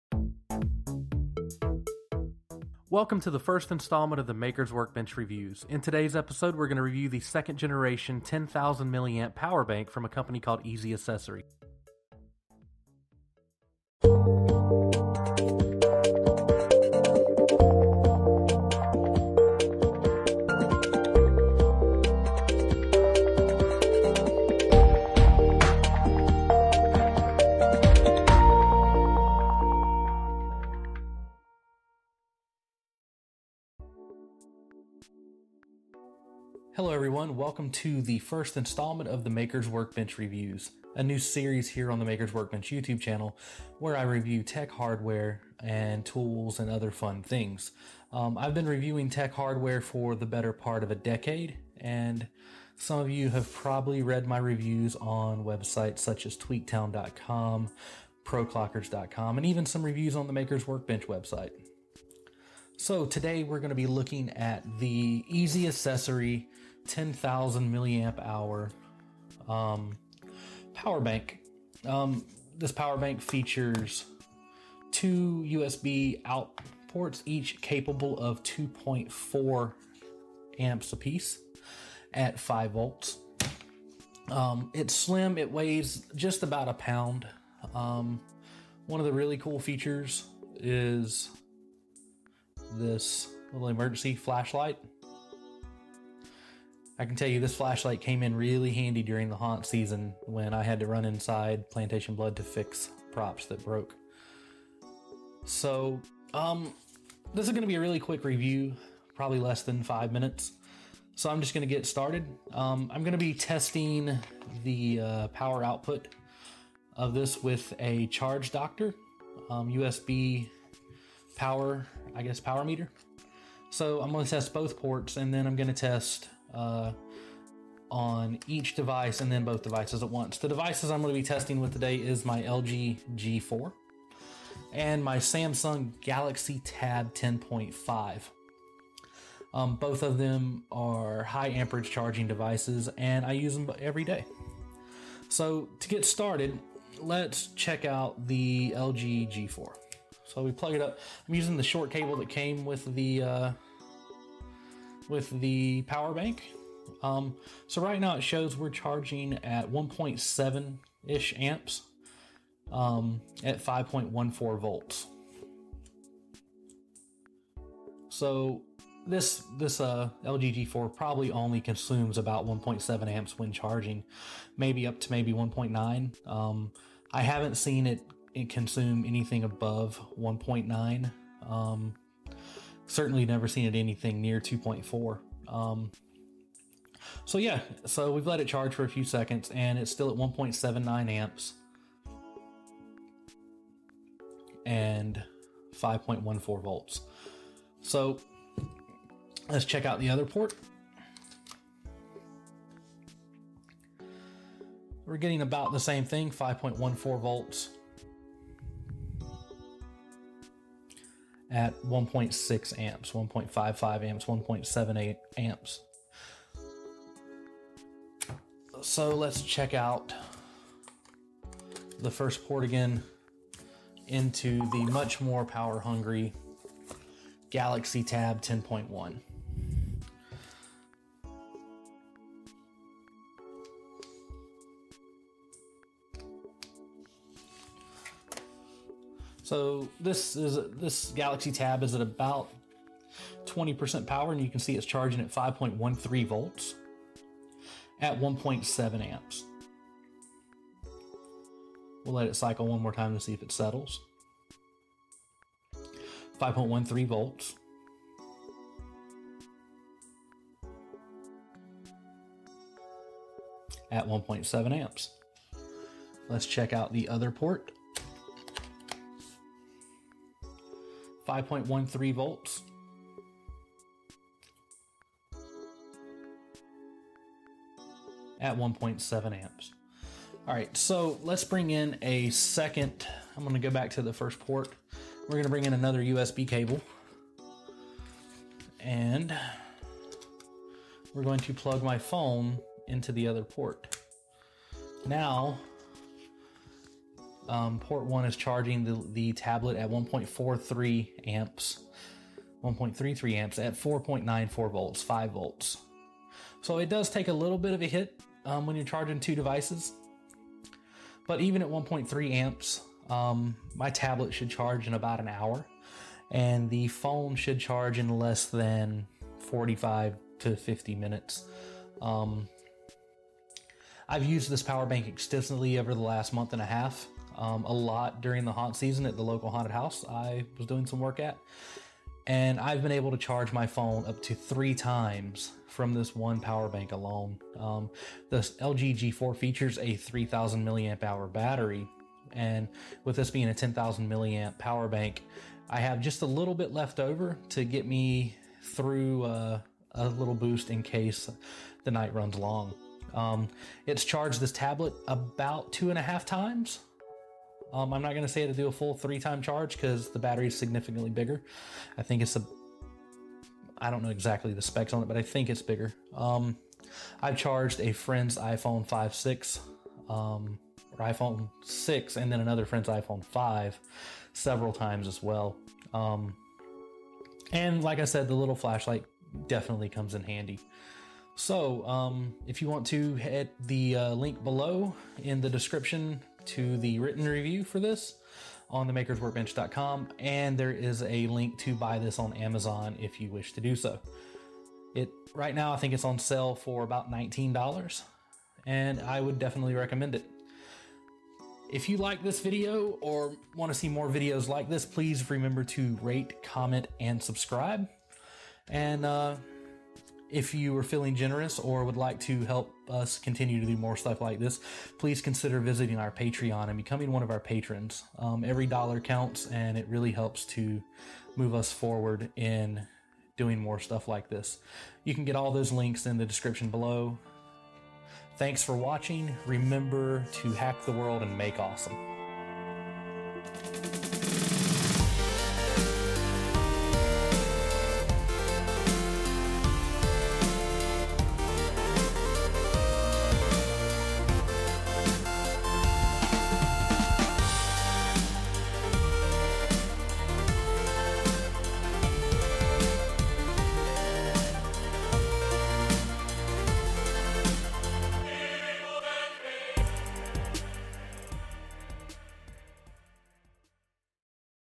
TheMakersWorkbench Reviews: EasyAcc 2nd Gen 10,000mAh Powerbank
This is the first review in a new series here at TheMakersWorkbench. ) to view the full written review, and find out why it scored a 98/100 when all things were said and done!

You can buy this power bank on Amazon.com